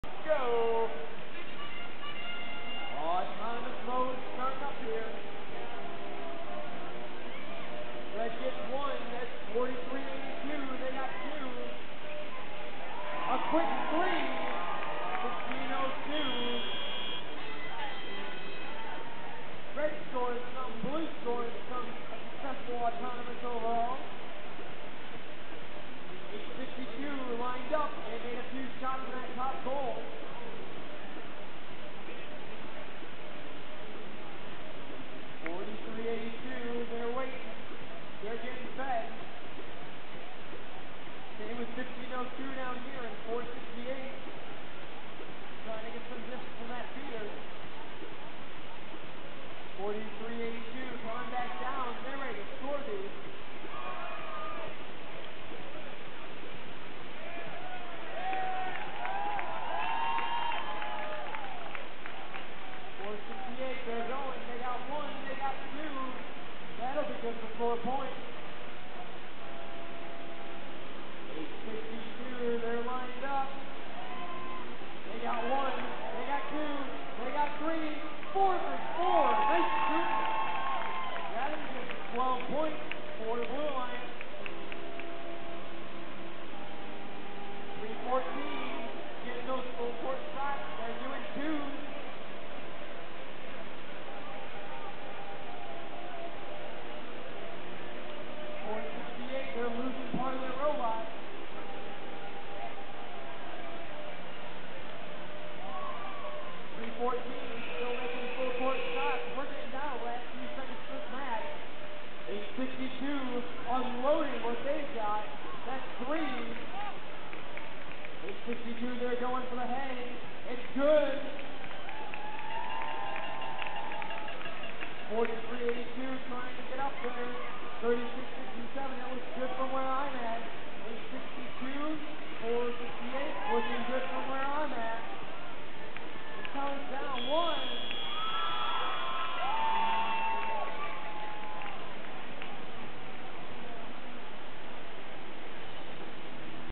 2013 FIRST Kettering District Qualification Match 49
Qualification Match 49 of the 2013 FIRST in Michigan Kettering District Competition
Red - FRC3667 FRC4382 FRC314
Blue - FRC862 FRC468 FRC1502
Score: Red 64 Blue 69